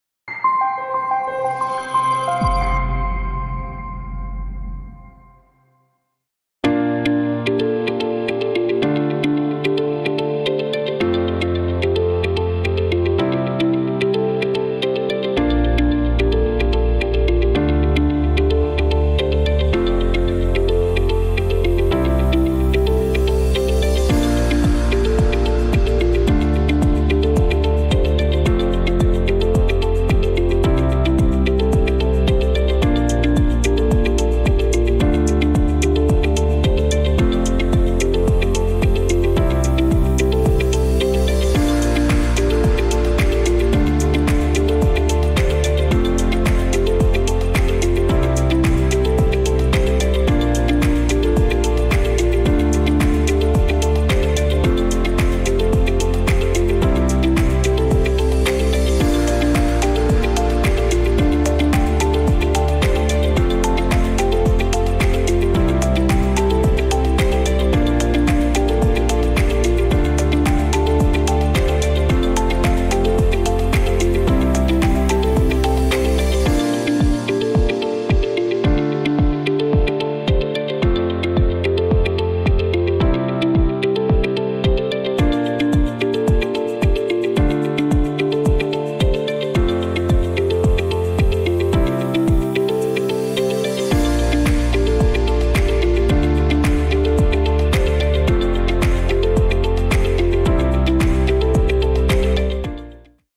How We Handmade Our Fiberglass Planter | YEWSENG GARDENING SUPPLY
Have you ever wondered how fiberglass pots are made? Join us for a quick tour of our pot manufacturing to find out!